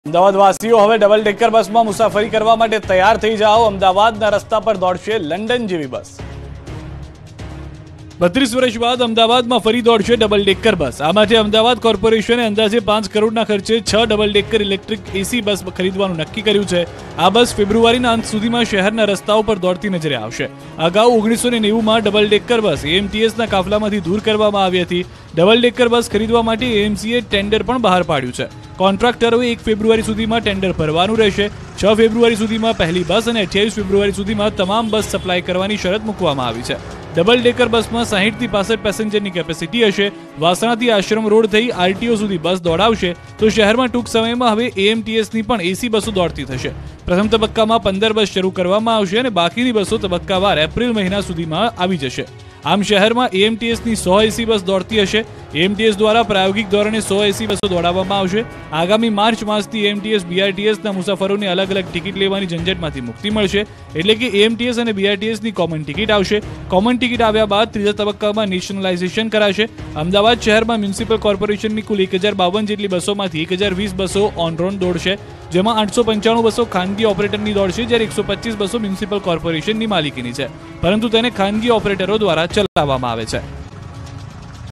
Ahmedabad News : અમદાવાદના રસ્તા પર દોડશે લંડન જેવી બસ | Double Decker Bus | Gujarati News | News18
Ahmedabadites get ready to travel in double decker bus now. London-like buses will run on the roads of Ahmedabad.

અમદાવાદીઓ હવે ડબલડેકર બસમાં મુસાફરી કરવા માટે તૈયાર થઈ જાઓ. અમદાવાદના રસ્તા પર દોડશે લંડન જેવી બસ. n18oc_ahmedabad